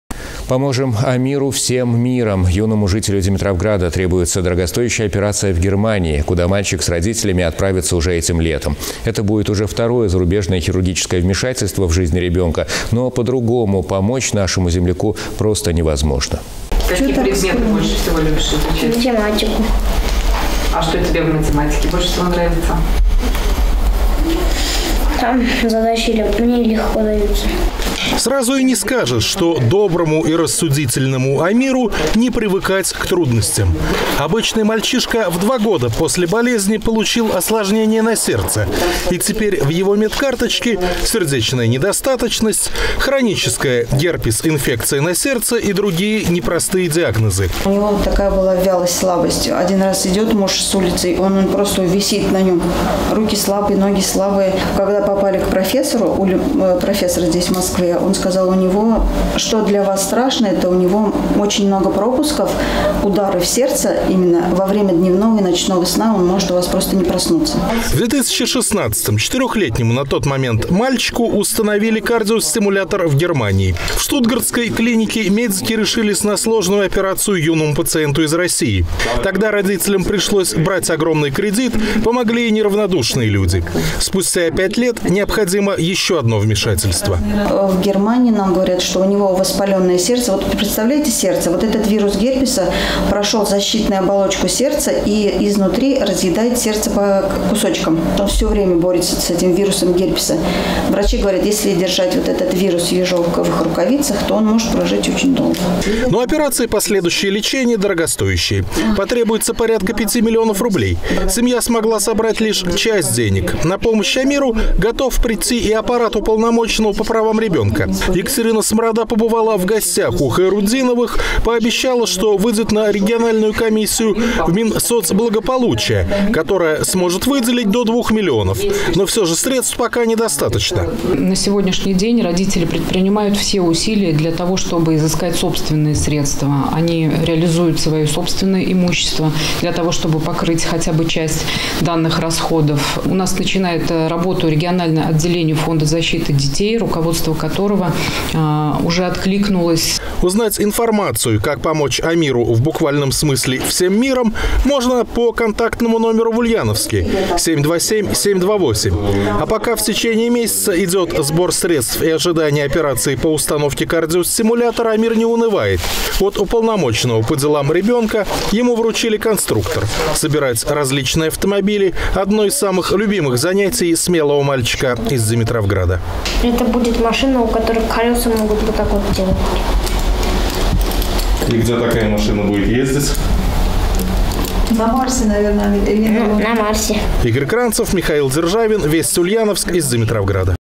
Юному жителю Димитровграда требуется дорогостоящая операция в Германии
Это будет уже второе зарубежное хирургическое вмешательство  в жизни ребёнка, но по-другому помочь нашему земляку просто невозможно.